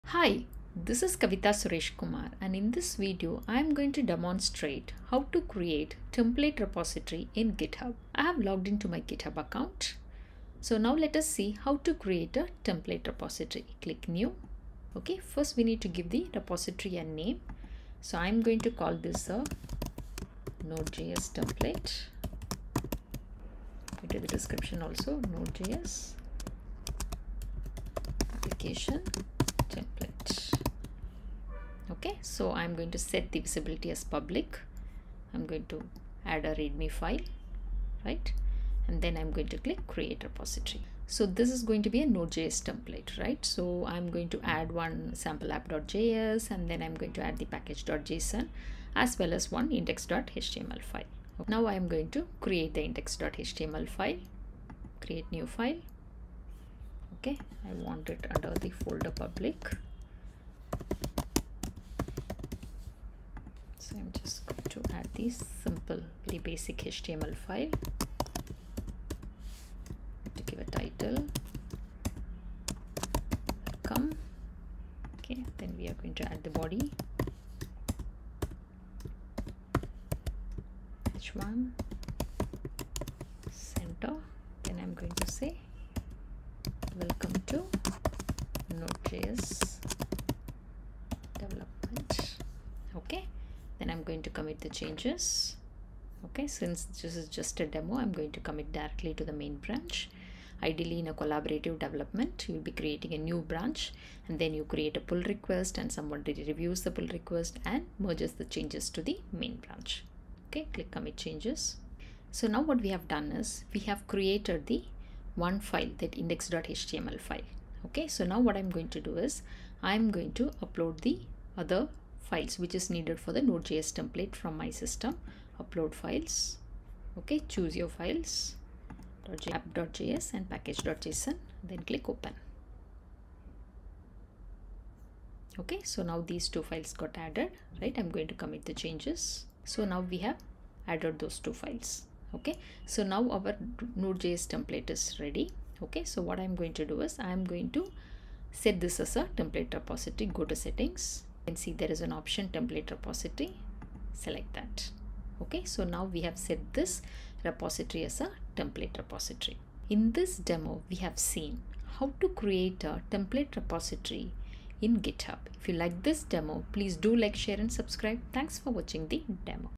GitHub: Creating Template Repository
In this video, you will learn how to create template repository in GitHub